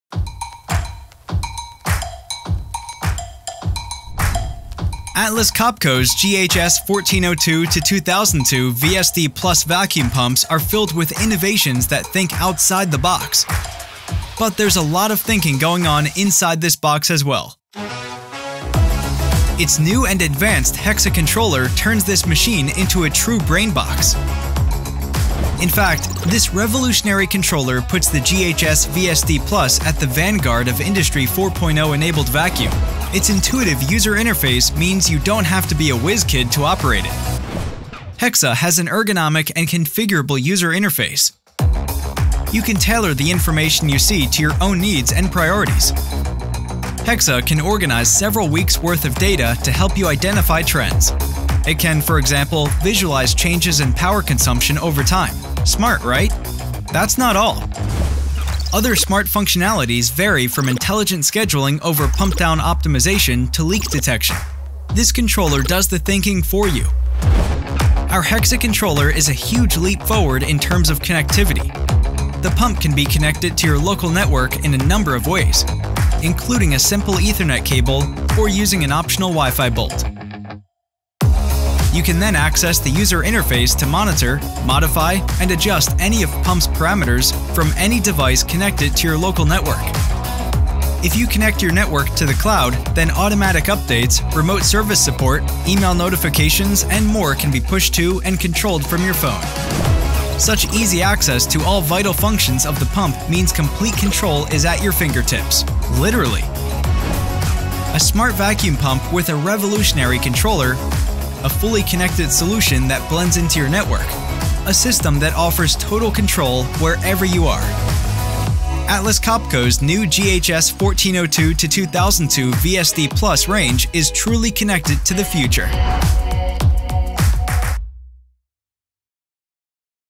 The industry 4.0 vacuum solution - The new GHS 1202-2002 VSD+
Atlas Copco’s GHS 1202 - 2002 VSD+ vacuum pumps are filled with innovations that think outside the box. But there’s a lot of thinking going on inside this box as well. Its new and advanced HEX@ controller turns this machine into a true brainbox.

In fact, this revolutionary controller puts the GHS VSD+ at the vanguard of Industry 4.0 enabled vacuum. And rest assured: its intuitive user interface means you don’t have to be a whiz kid to operate it.

HEX@ has an ergonomic and configurable user interface. You can tailor the information you see to your own needs and priorities.

HEX@ can organize several weeks’ worth of data to help you identify trends. It can for example visualize changes in power consumption over time. Smart, right?

That’s not all. Other smart functionalities vary from intelligent scheduling over pump-down optimization to leak detection. This controller does the thinking for you.

Our HEX@ controller is a huge leap forward in terms of connectivity. The pump can be connected to your local network in a number of ways, including a simple ethernet cable or using an optional Wi-Fi bolt.

You can then access the user interface to monitor, modify and adjust any of pump’s parameters from any device connected to your local network.

If you connect your network to the cloud, then automatic updates, remote service support, email notifications and more can be pushed to and controlled from your phone.

Such easy access to all vital functions of the pump means complete control is at your fingertips... literally.

A smart vacuum pump with a revolutionary controller. A fully connected solution that blends into your network. A system that offers total control, wherever you are.
Atlas Copco’s new GHS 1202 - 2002 VSD+ range is truly connected to the future.